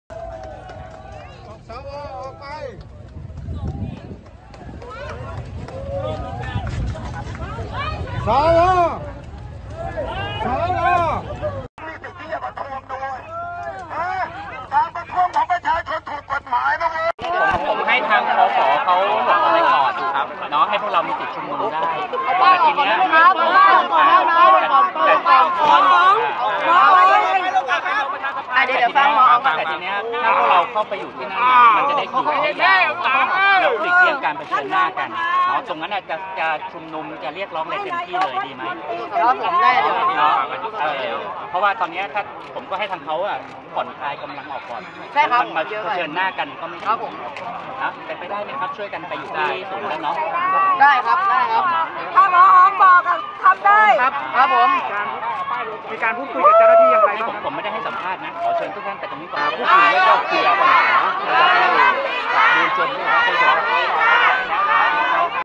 ม็อบส้มเดือดจุดพลุแฟร์-โปรยใบลาออก ส.ว.ประชิดรั้วสภา “ปดิพัทธ์” รุดเคลียร์ “เจี๊ยบ” แจมขอเปิดประตู
ด้อมส้มเดือด!!! จุดพลุแฟร์-โปรยใบลาออกให้ ส.ว. ทำพิธีขับไล่หน้าสภา หลังศาล รธน.สั่ง “พิธา” หยุดปฏิบัติหน้าที่ แห่กรูโวยประชิดรั้ว “เจี๊ยบ” โผล่ขอ จนท.เปิดประตู “ปดิพัทธ์” ต้องเข้ามากล่อมม็อบ